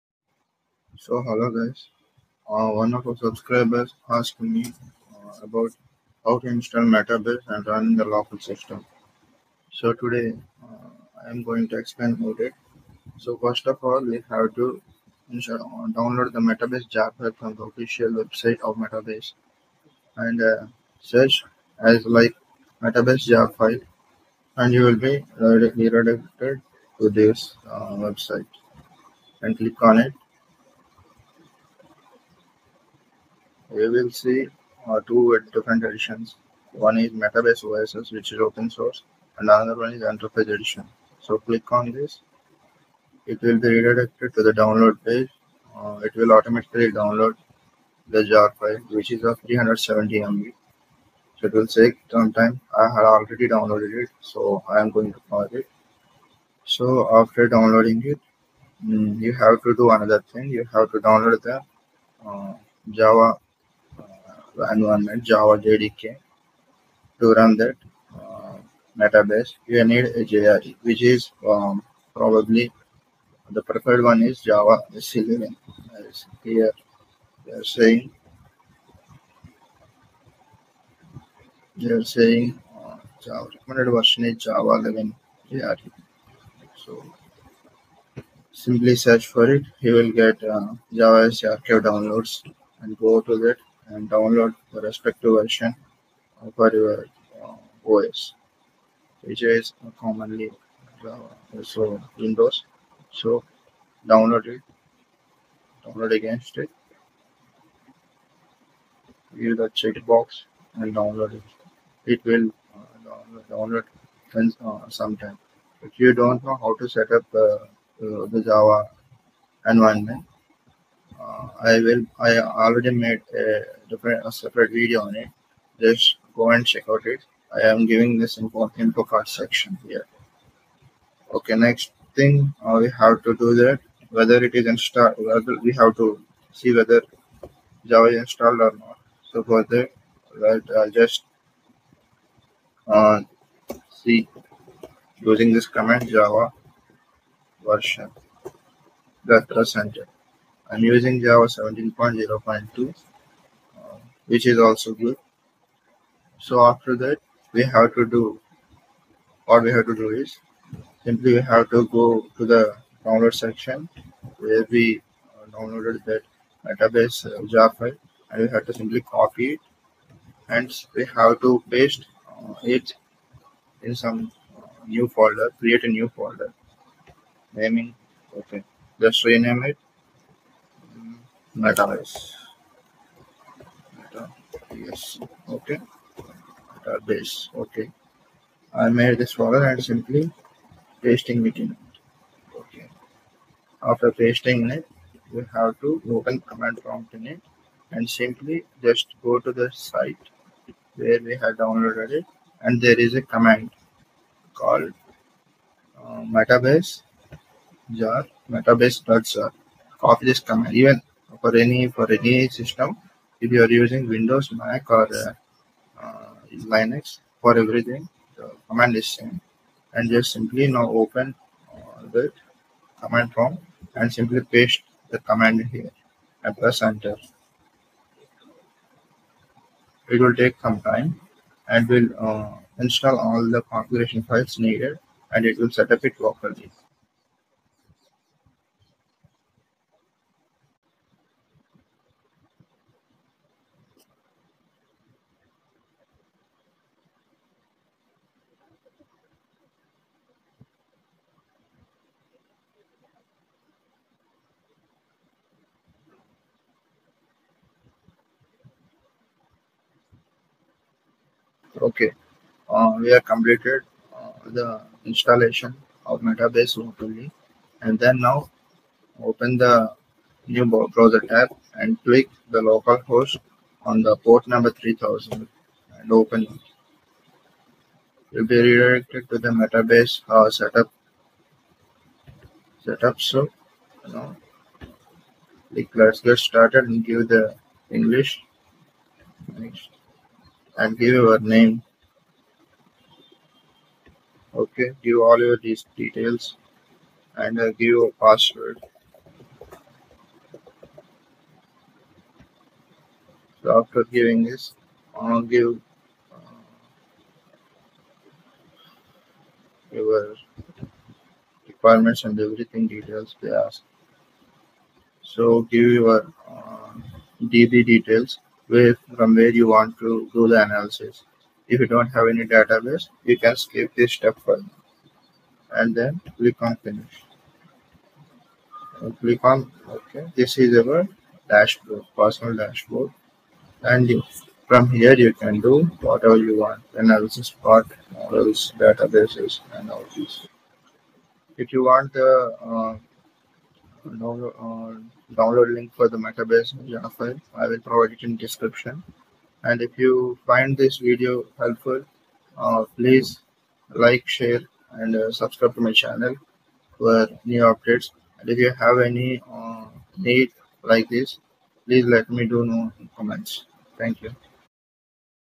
Installation of Metabase Using Jar file in local environment||Full Explanation Video
command: java -jar metabase.jar
use this command in the directory where you placed metabase jar file.

Video Tutorial on how to download and install jdk and setup jre environment in your system